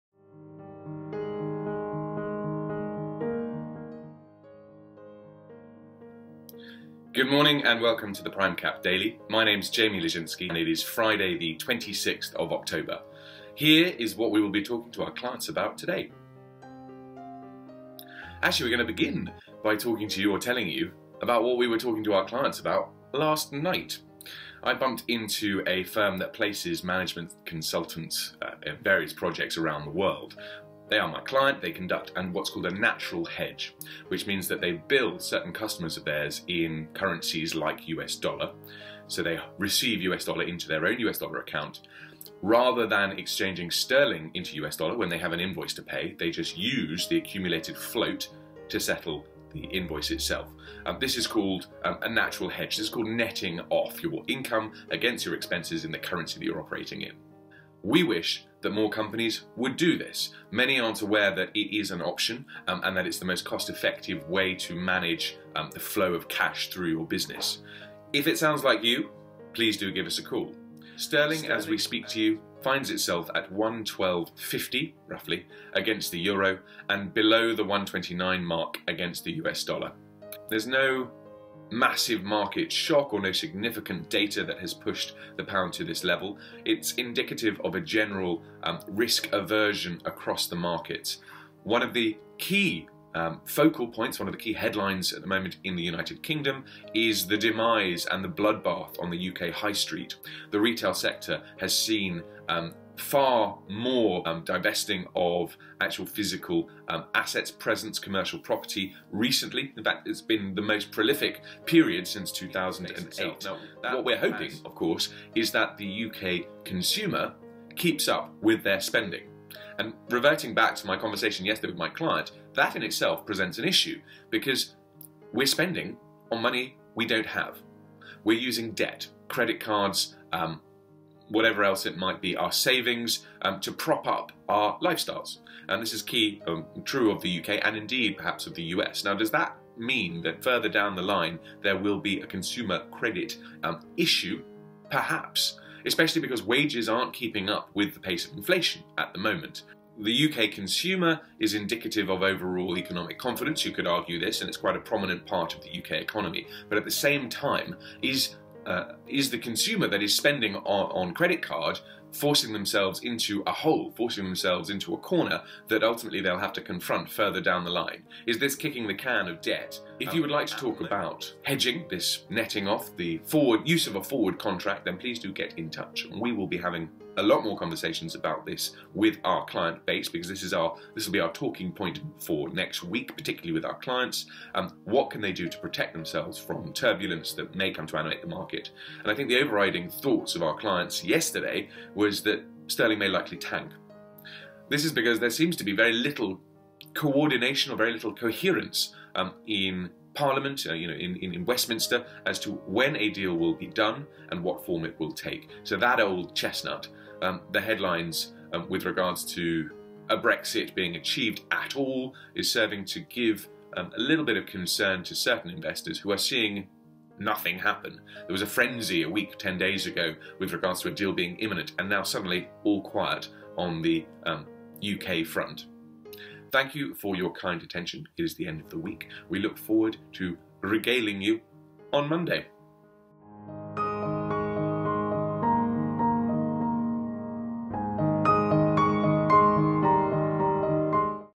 The Prime Cap Daily - 26th Oct 2018
Talking points for Friday 26th October: GBP vs EUR at 1.1250. UK retail bloodshed.  Inflation and consumer debt.  Facebook